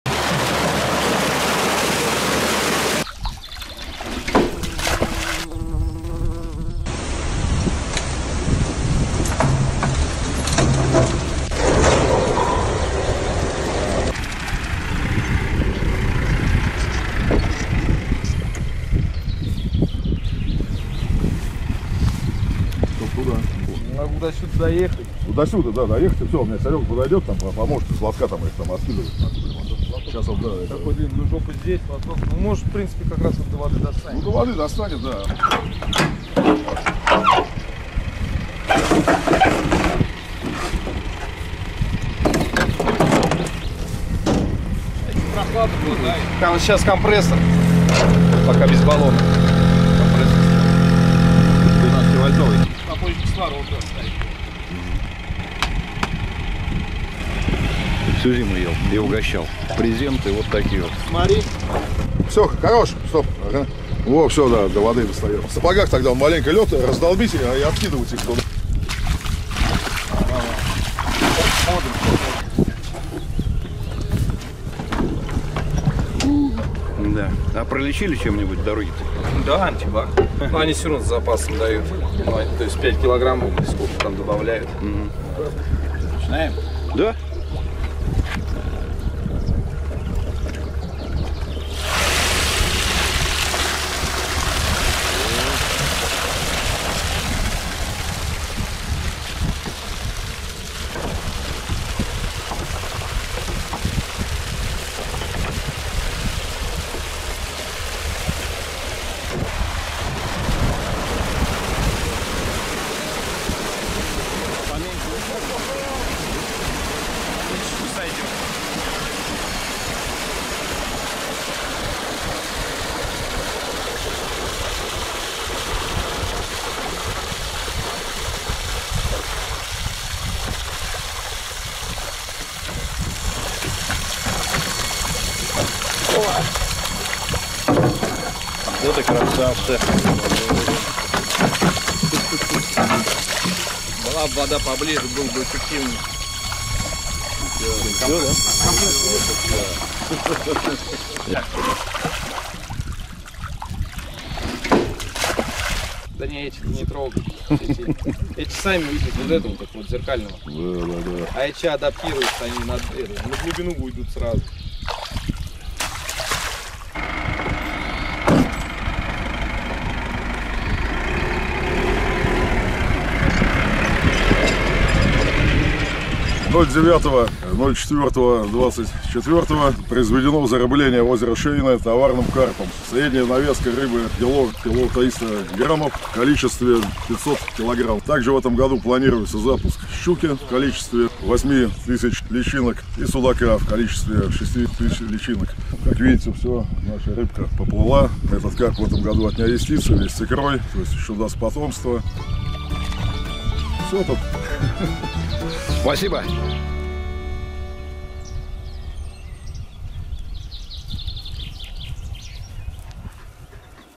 Озеро Шейно. Зарыбление большим карпом 2024
Первое зарыбление озера в 2024 году. Крупный карп.

Помочь каналу очень просто:
Поставь 👍 или 👎 видеоролику
Напиши комментарий под видео